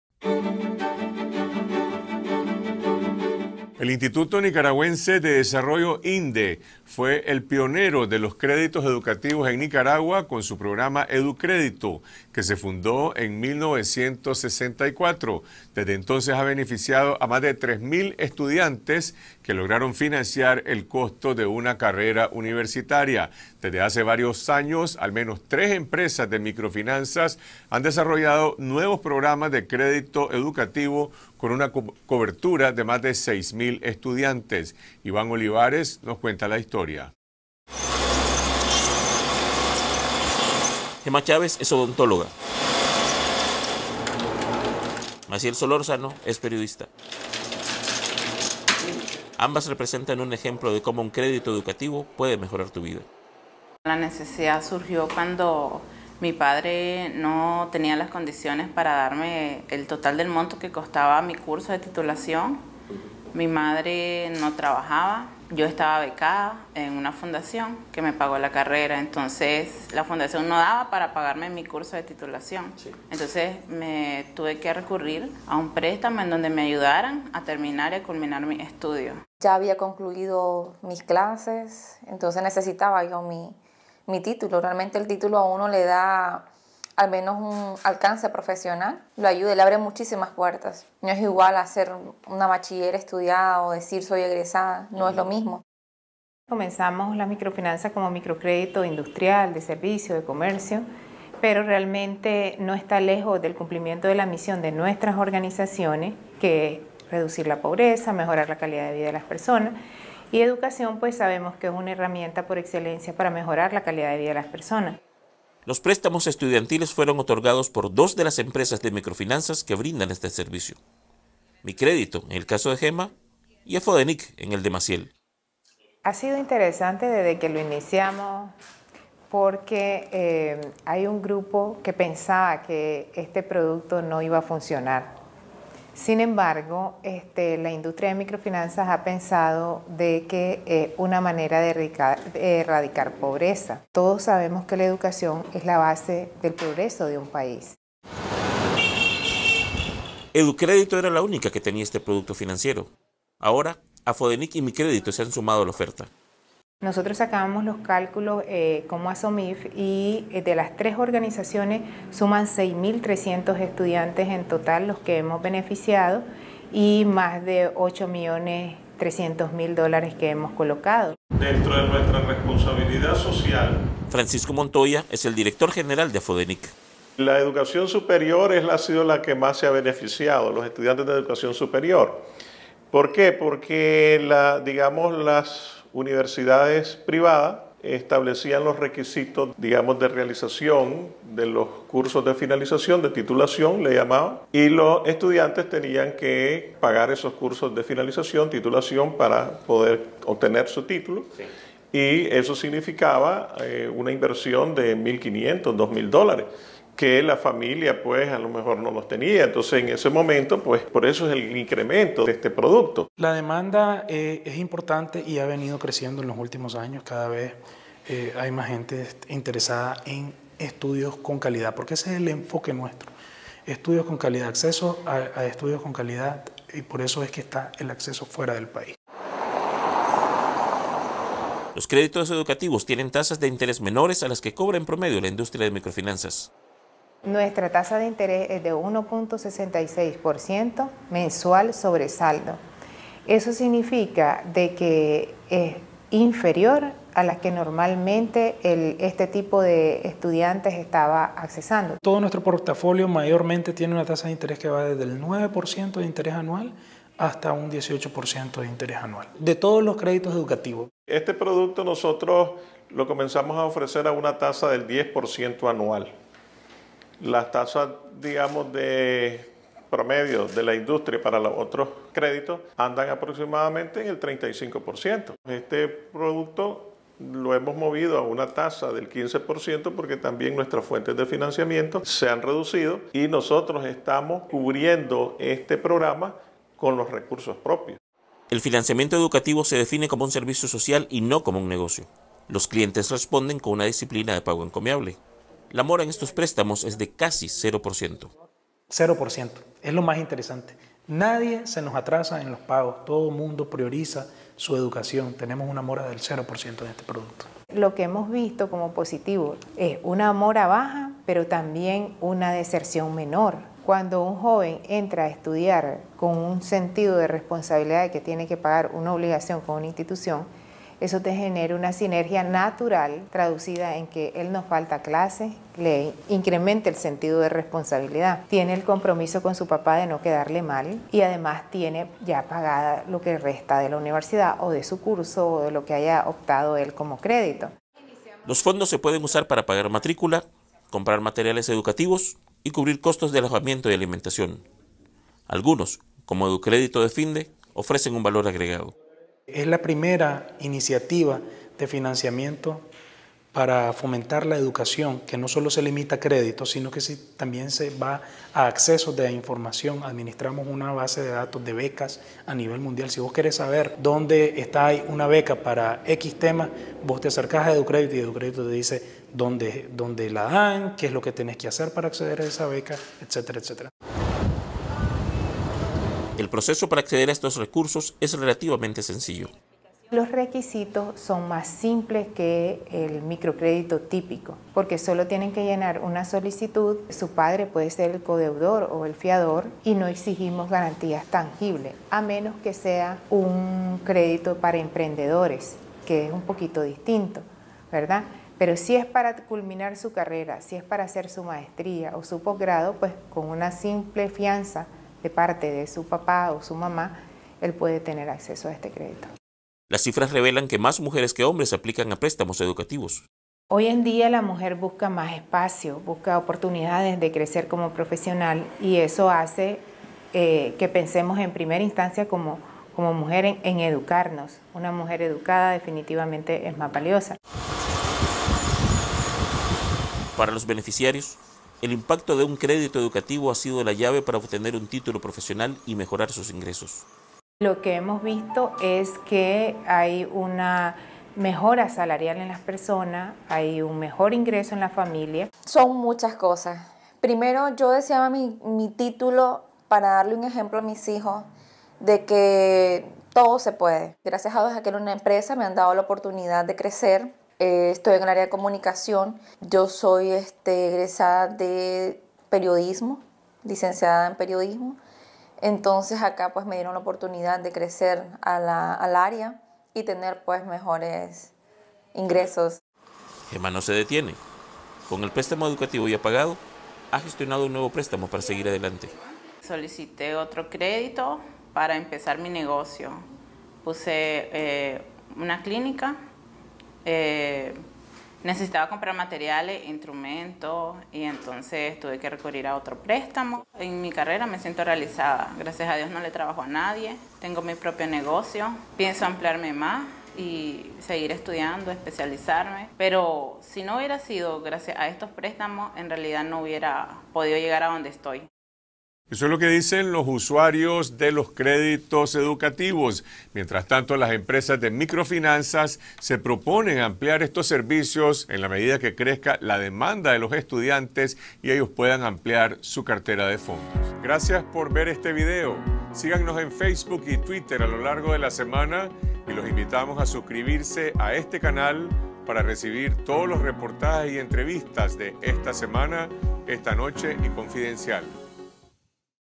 Empresas de microfinanzas apuestan a prestamos estudiantiles
Tres empresas de microfinanzas se enfocan en préstamos estudiantiles, para favorecer el acceso a la educación superior.